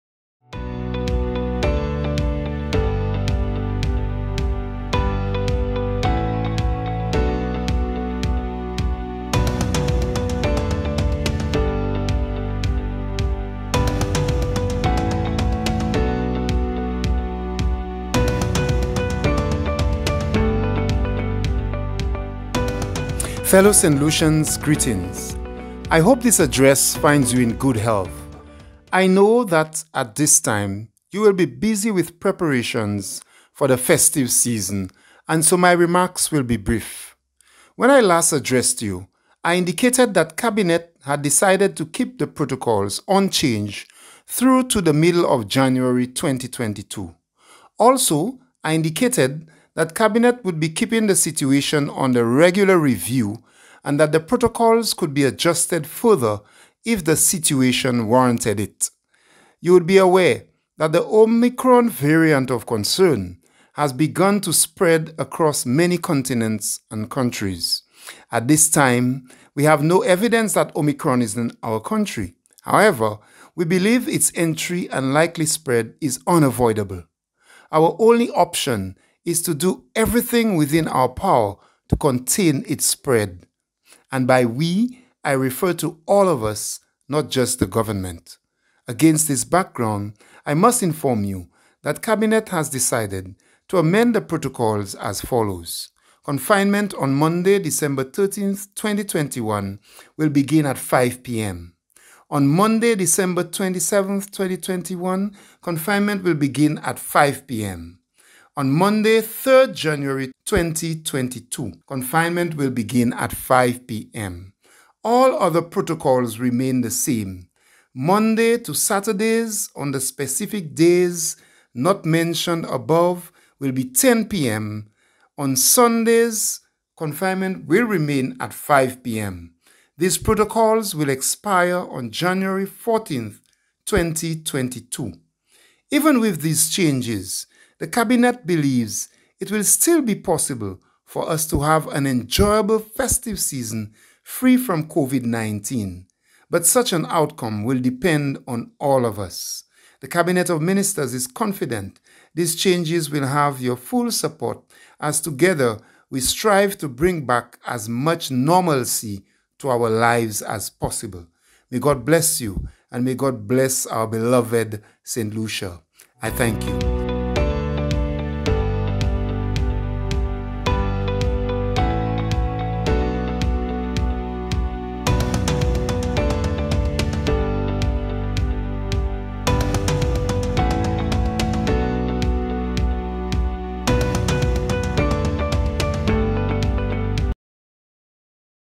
Amended COVID-19 Protocols for December 2021 and January 2022
The Omicron Variant of Concern has begun to spread across many continents and countries. At this time, we have no evidence that Omicron is in our country. However, we believe its entry and likely spread is unavoidable. Against this background, I must inform you that Cabinet has decided to amend the protocols as follows:
 Confinement on Monday, December 13, 2021 will begin at 5 p.m.
 Confinement on Monday December 27, 2021 will begin at 5 p.m.
 Confinement on Monday January 3, 2021 will begin at 5 p.m.

All other protocols remain the same. Monday to Saturdays on the specific days not mentioned above will be 10 p.m. for confinement. On Sundays, confinement will remain at 5 p.m. These protocols will expire on January 14, 2021.